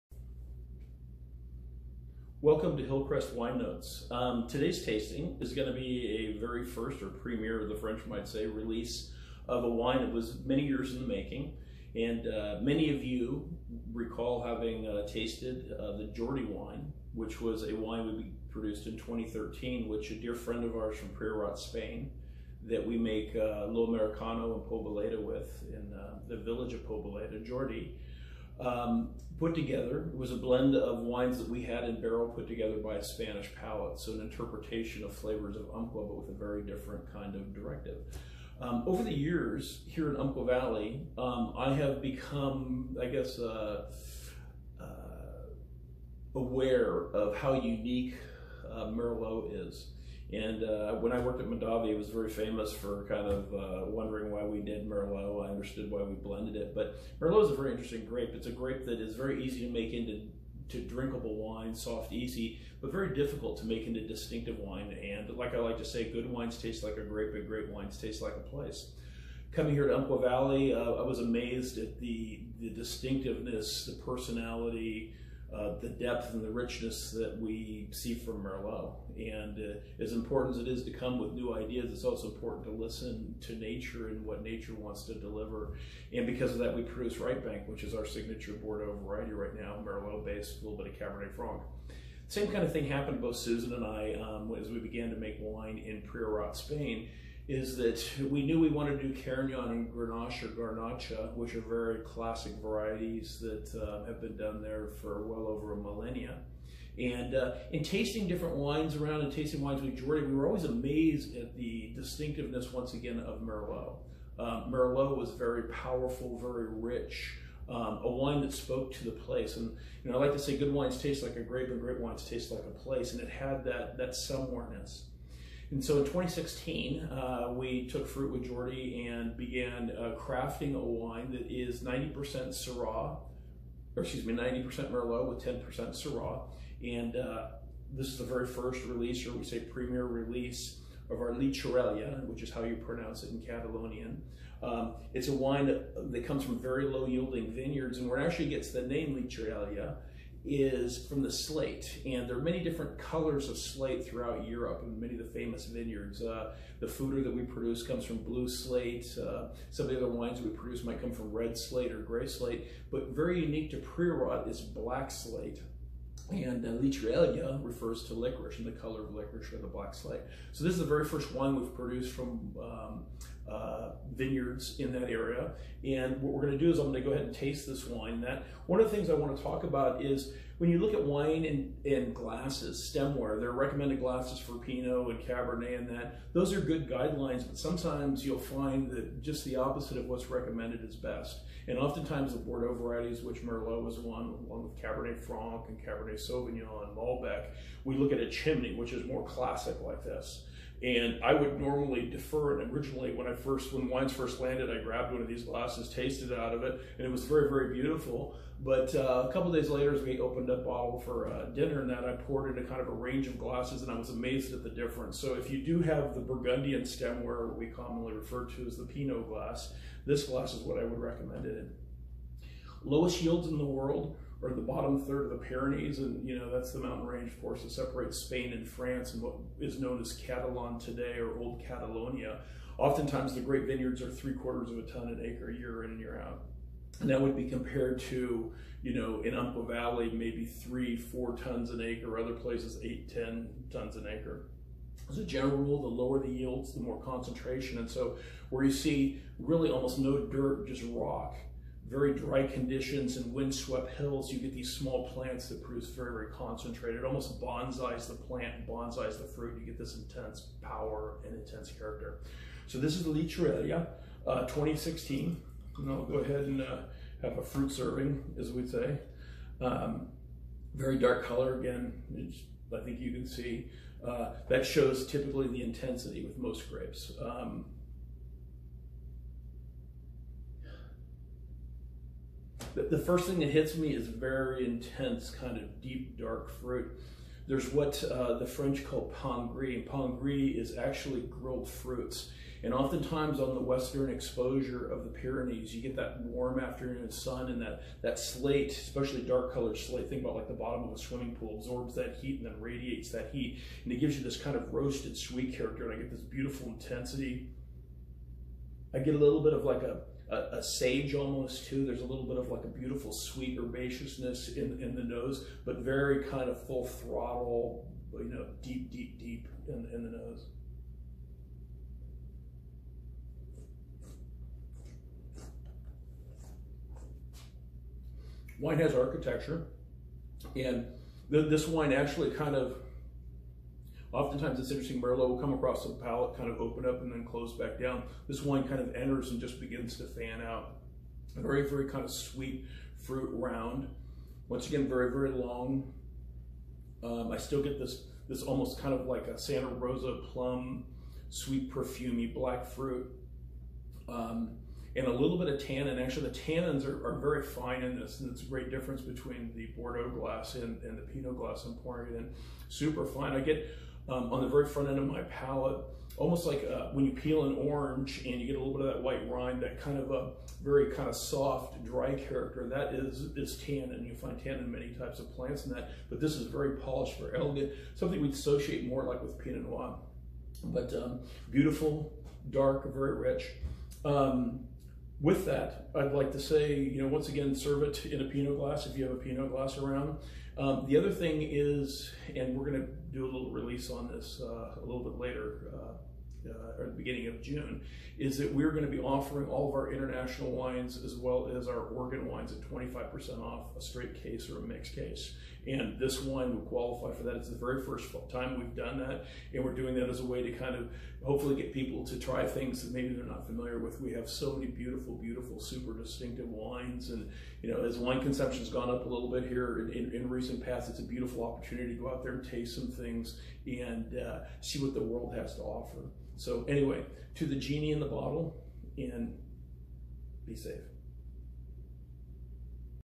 Wine notes 2016 Llicorella
Wine tasting notes for our first release of a Merlot/Syrah blend from Priorat, Spain